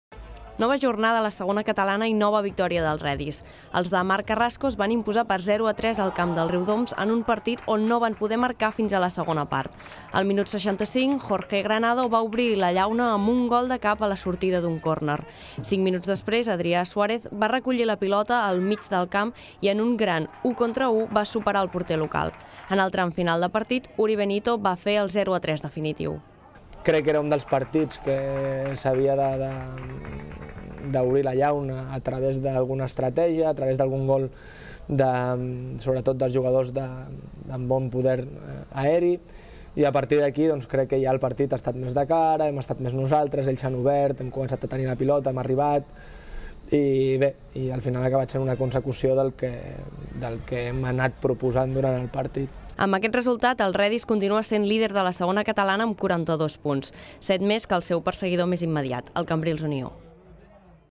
Desena jornada consecutiva sense perdre del CF Reddis
El Reddis continua en estat de gràcia. La victòria a Riudoms (0-3) i les ensopegades dels seus perseguidors, deixen els roigs-i-negres com a líders de la Segona Catalana amb set punts d'avantatge respecte al Cambrils Unió.